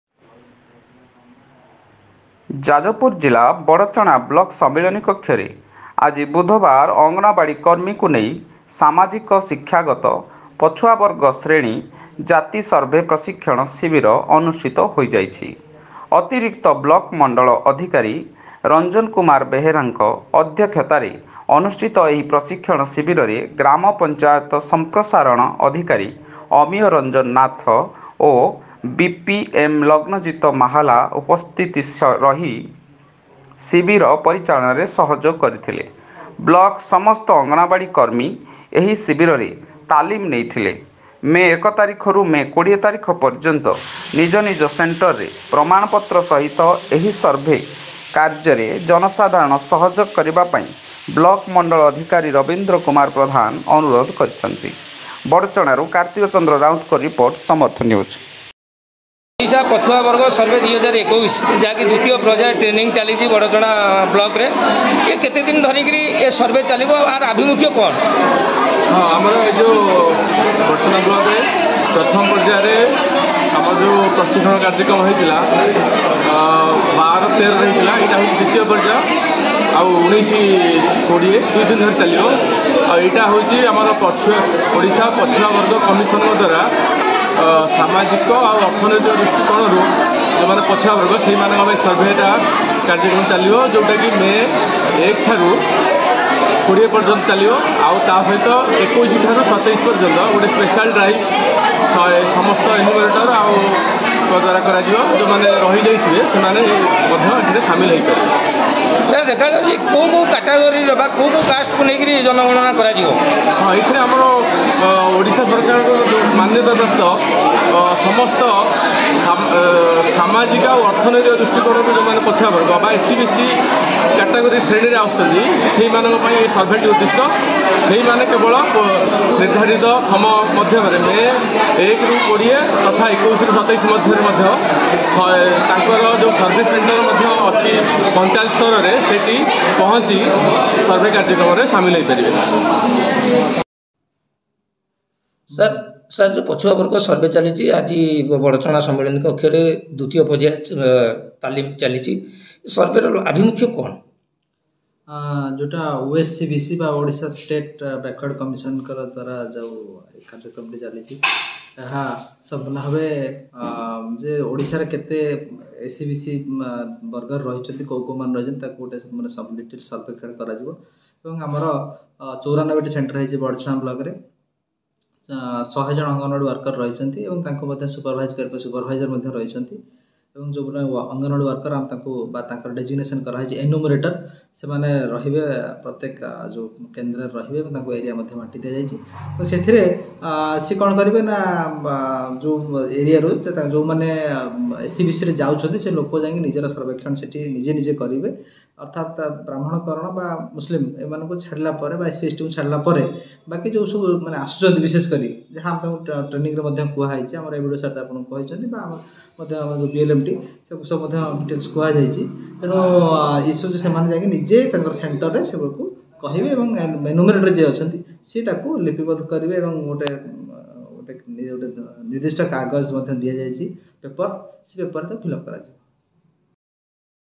News Update:ଓଡ଼ିଶା ପଛୁଆ ବର୍ଗ ସର୍ଭେ ୨୦୨୧ ପ୍ରଶିକ୍ଷଣ ଶିବିର ଅନୁଷ୍ଠିତ ହୋଇଅଛି
ଯାଜପୁର ଜିଲ୍ଲା ବଡ଼ଚଣା ବ୍ଲକ ସମ୍ମିଳନୀ କକ୍ଷରେ ଆଜି ବୁଧବାର ଅଙ୍ଗନୱାଡି କର୍ମୀଙ୍କୁ ନେଇ ସାମାଜିକ ଶିକ୍ଷାଗତ,ପଛୁଆବର୍ଗ ଶ୍ରେଣୀ ଜାତି ସର୍ଭେ ପ୍ରଶିକ୍ଷଣ ଶିବିର ଅନୁଷ୍ଠିତ ହୋଇଯାଇଛି I ଅତିରିକ୍ତ ବ୍ଲକ ମଣ୍ଡଳ ଅଧିକାରୀ ରଞ୍ଜନ କୁମାର ବେହେରାଙ୍କ ଅଧ୍ୟକ୍ଷତାରେ ଅନୁଷ୍ଠିତ ଏହି ପ୍ରଶିକ୍ଷଣ ଶିବିରରେ ଗ୍ରାମପଞ୍ଚାୟତ ସମ୍ପ୍ରସାରଣ ଅଧିକାରୀ ଅମୀୟ ରଞ୍ଜନ ନାଥ ଓ ବିପି ଏମ ଲଗ୍ନଜିତ ମାହାଲା ଉପସ୍ଥିତ ରହି ଶିବିର ପରିଚାଳନାରେ ସହଯୋଗ କରିଥିଲେ I ବ୍ଲକର ସମସ୍ତ ଅଙ୍ଗନୱାଡି କର୍ମୀ ଏହି ଶିବିରରେ ତାଲିମ ନେଇଥିଲେ I ମେ ଏକ ତାରିଖରୁ  ମେ କୋଡିଏ ତାରିଖ ପର୍ଯ୍ୟନ୍ତ ନିଜ ନିଜ ସେଣ୍ଟରରେ ପ୍ରମାଣପତ୍ର ସହିତ ଏହି ସର୍ଭେ କାର୍ଯ୍ୟରେ ଜନସାଧାରଣ ସହଯୋଗ କରିବାପାଇଁ  ବ୍ଲକ ମଣ୍ଡଳ ଅଧିକାରୀ ରବୀନ୍ଦ୍ର କୁମାର ପ୍ରଧାନ ଅନୁରୋଧ କରିଛନ୍ତି I